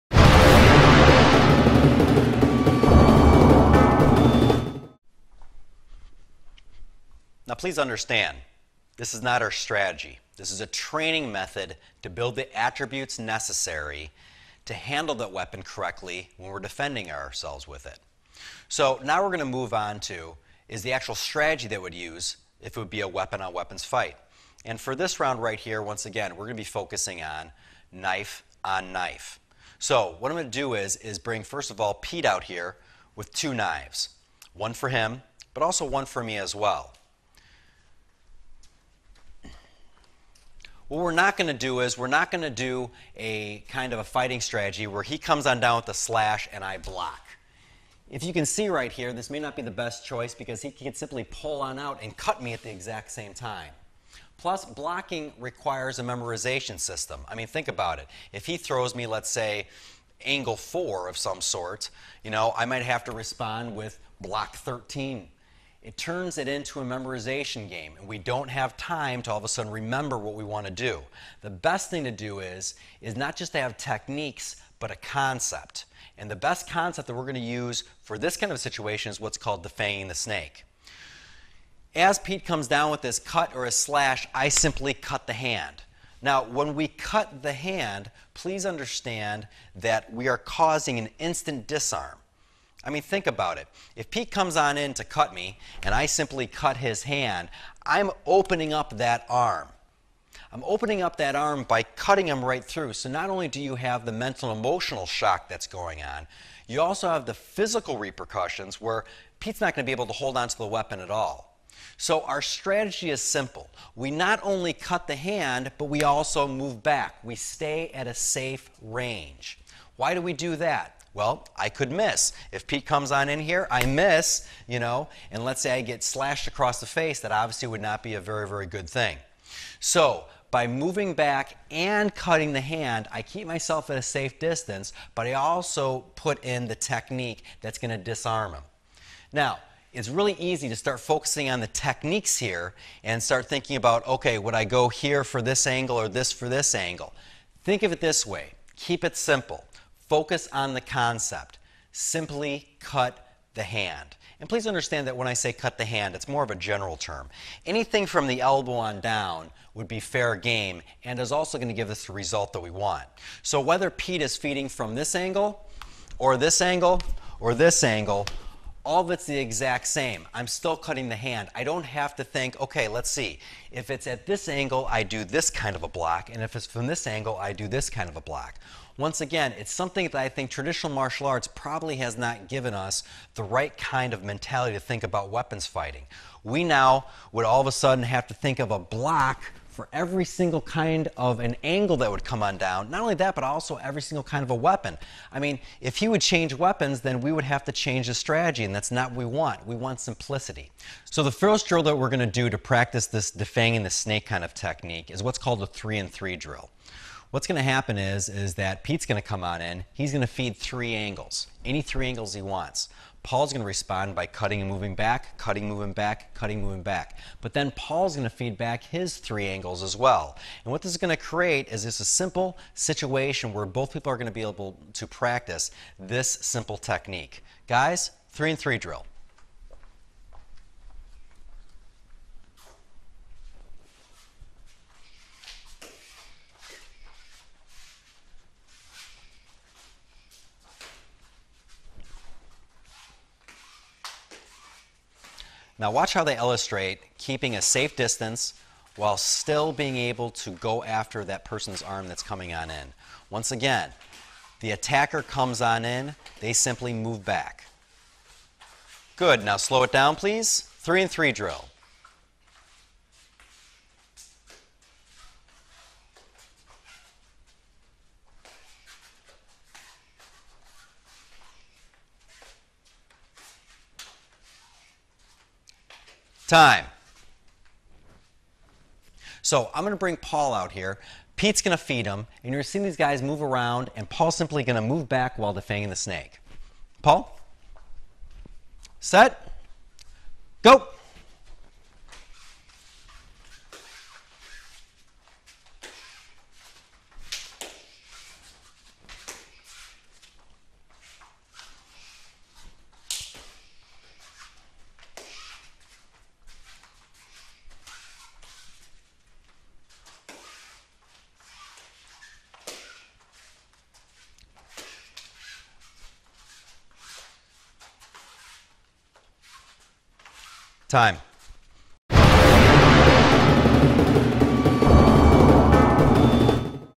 Knife-on-Knife Combat! - "Un-Fanging the Snake" Concept
--- Free Exclusive Knife Defenese Course

World Famous Self Defense Expert, Jerry Wetzel, Demonstrates 3 Simple Techniques That "Everyday People" Can Learn To Stay Safe On The Streets.

--- Free Exclusive Knife Defense Course